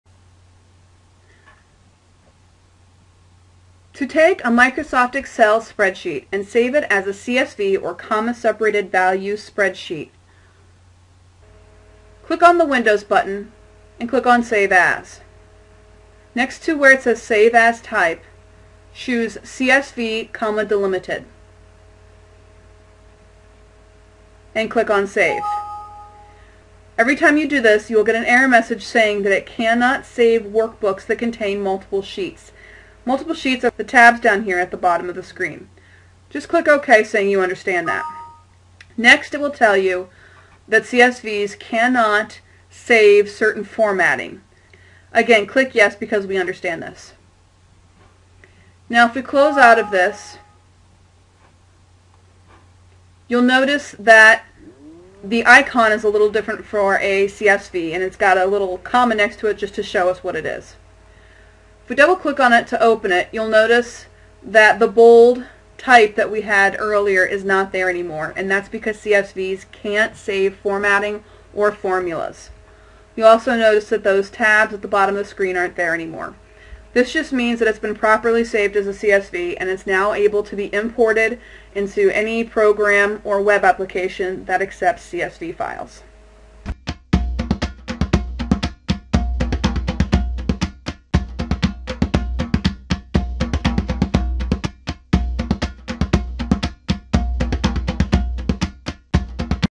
Saving Files Excel XLS Files as CSV
How to save an Excel XLS or XLSX spreadsheet to a CSV (comma delimited) file.